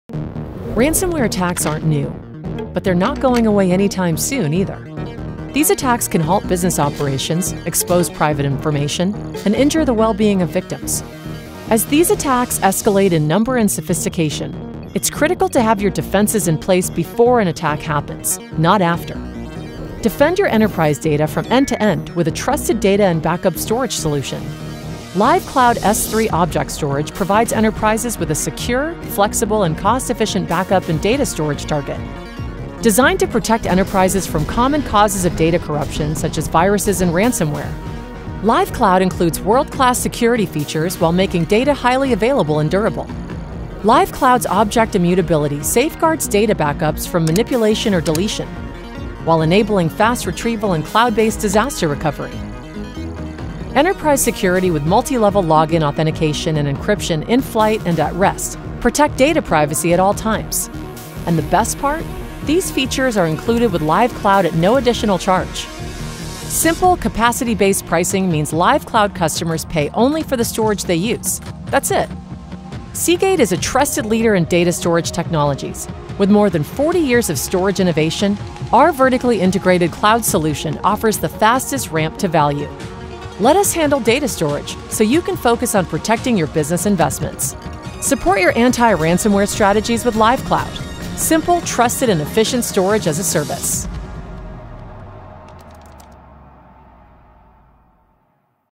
Seagate | Object Immutability for Ransomware Protection Use Case
Ransomware attacks can halt business operations, expose private information, and injure the well-being of victims—and they’re growing in number and sophistication. Safeguard enterprise backup data from cyberattacks and accidental manipulation or deletion with Seagate Lyve Cloud, cost-effective object storage that supports your anti-ransomware strategies.

Enterprise backup data is vulnerable to all kinds of ransomware attacks.
Seagate Lyve Cloud provides a secure, flexible, and cost-efficient backup storage target.
Enterprise security with multi-level login authentication and encryption in flight and at rest protect data privacy.
Lyve Cloud’s best-in-class security features are included with no additional charge.